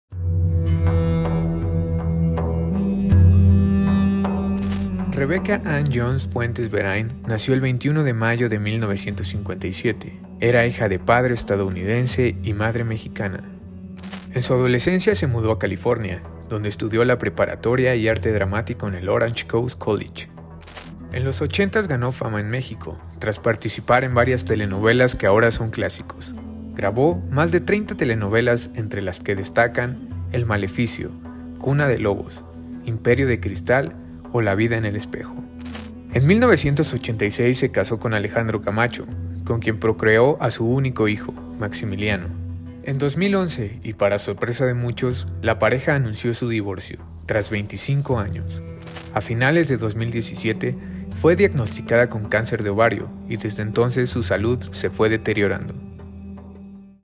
Rebecca Jones: La gran protagonista
Rebecca Jones, actriz y productora mexicana, murió a los 65 años dejando un legado en la televisión mexicana, así como también en el cine y el teatro con varias de sus obras, además de eso obtuvo bastantes nominaciones por sus actuaciones.